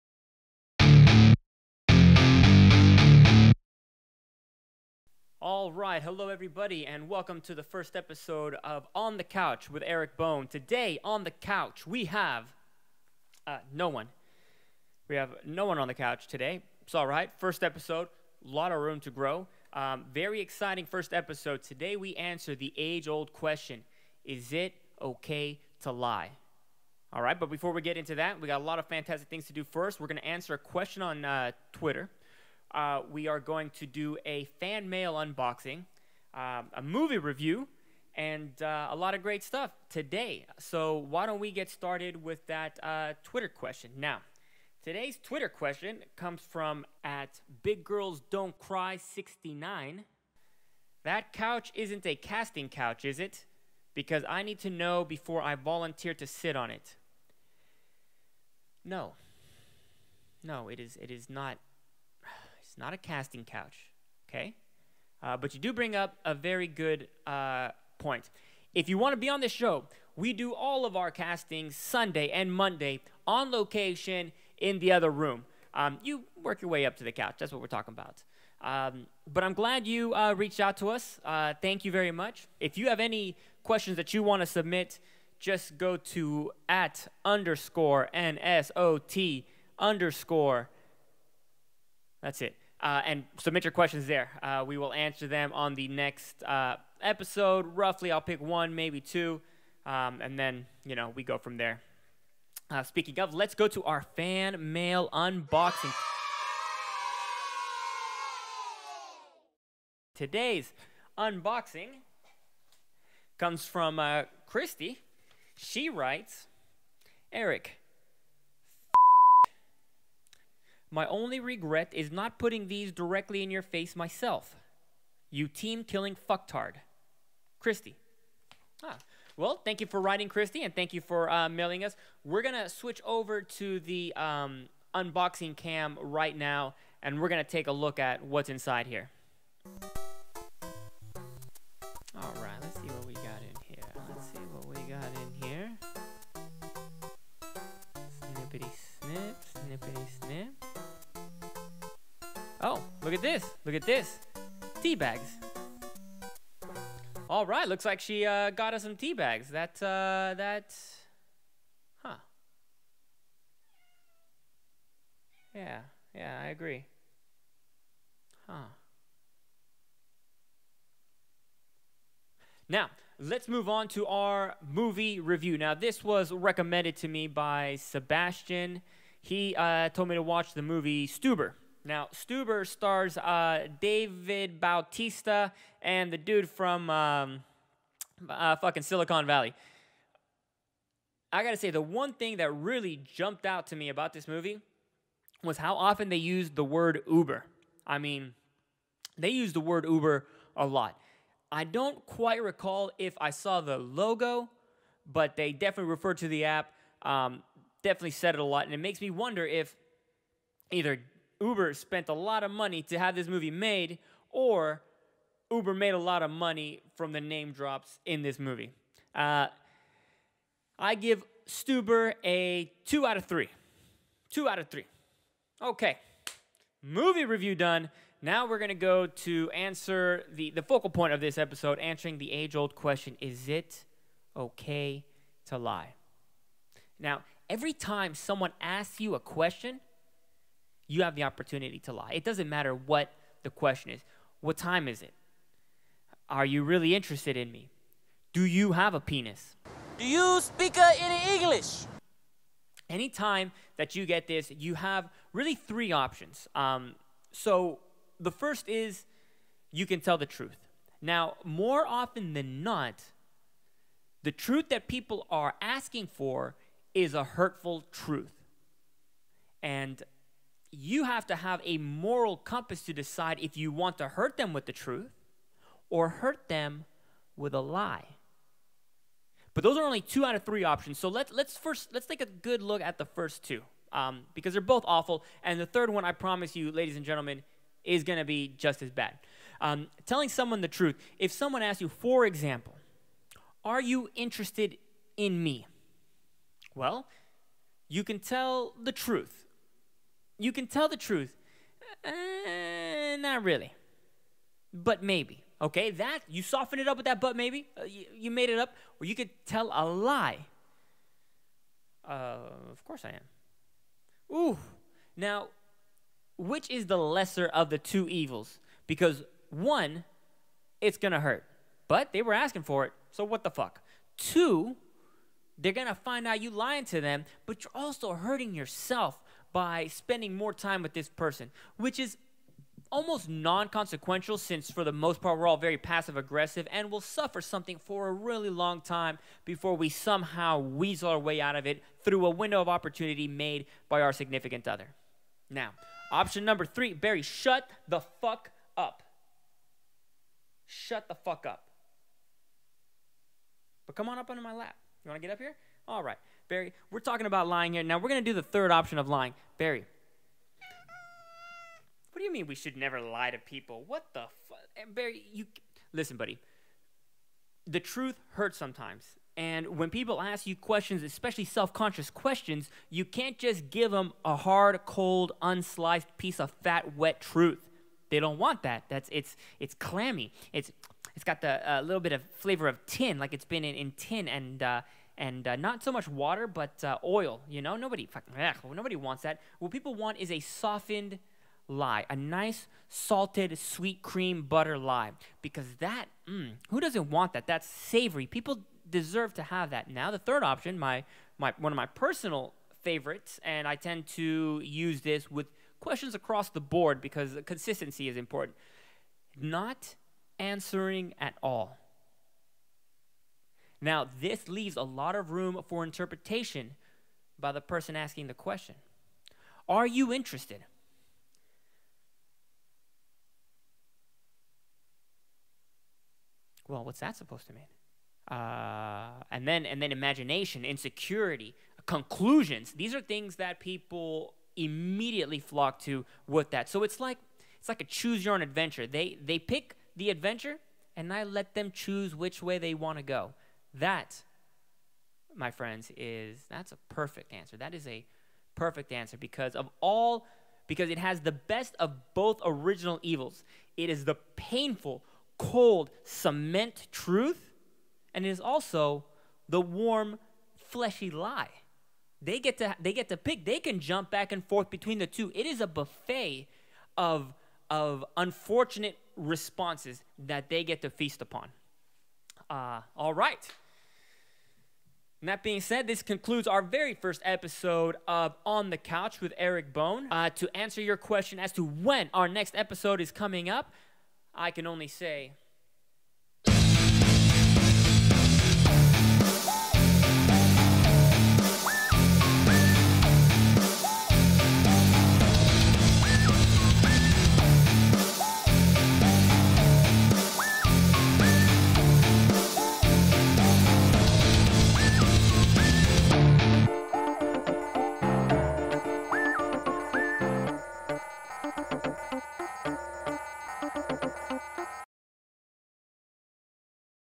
On The Couch ep1: Is it okay to lie?
In this episode we answer a twitter question, do a fan unboxing and review the movie Stuber!